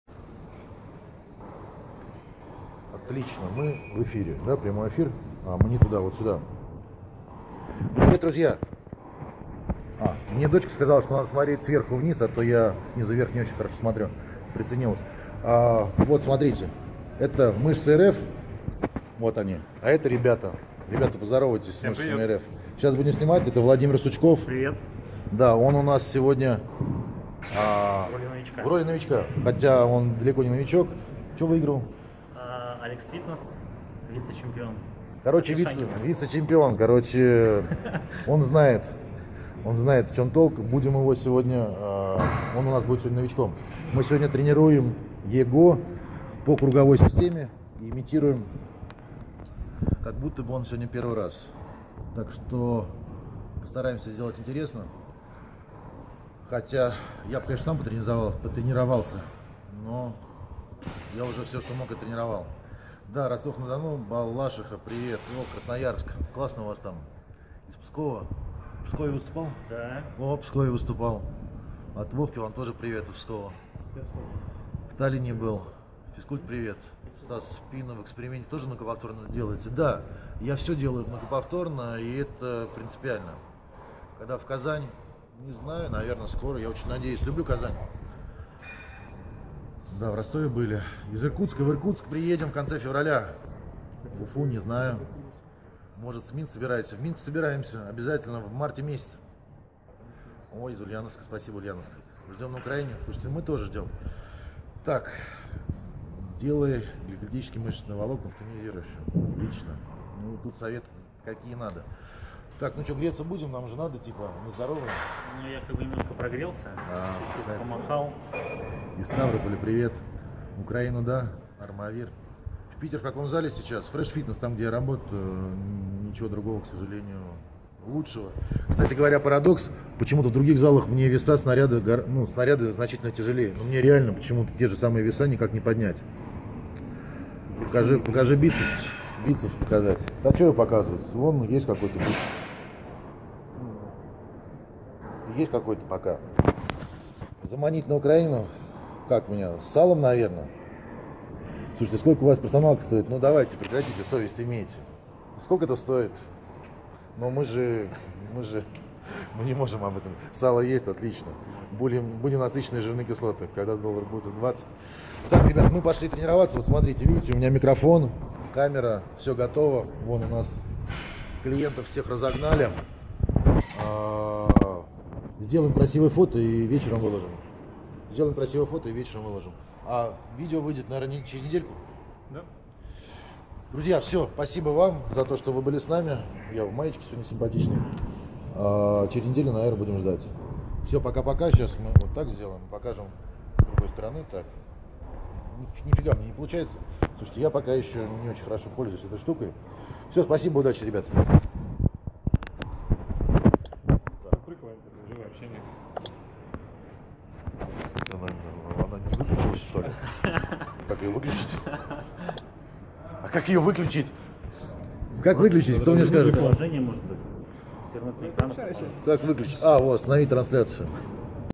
Стас Линдовер Качаем Мышцы РФ
Стас Линдовер в перерывках между сьемками нашел время поперископить